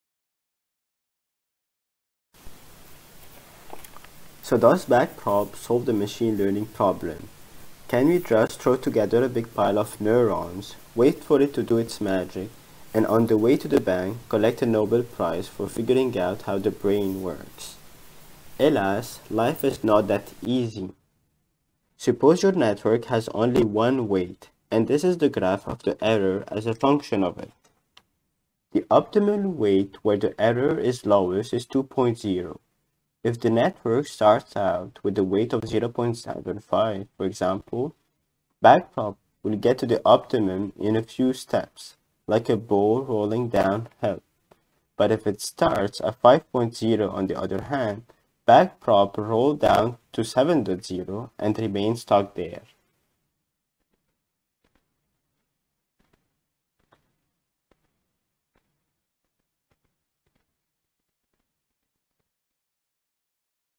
AudioVision: Real-time Speech Separation, Noise Suppression & Speaker Recognition
First try at solving the "cocktail party" problem